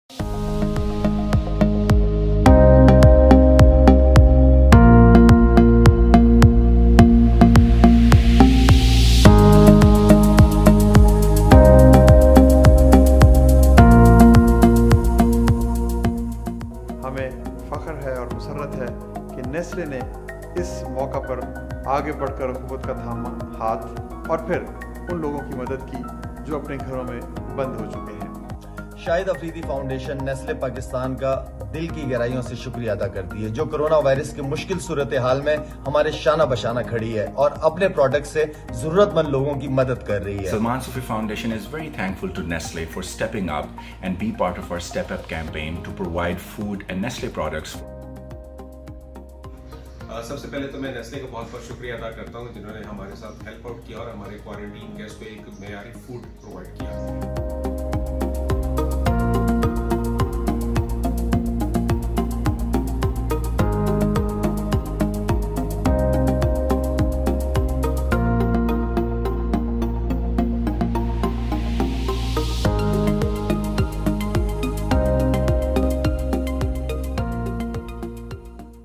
Nestlé Pakistan donated PKR 100 million to support vulnerable communities
Nestlé Pakistan has fulfilled the commitment of PKR 100 million to meet the nutritional needs of the vulnerable communities. We are committed in continuing our efforts in contributing to our society.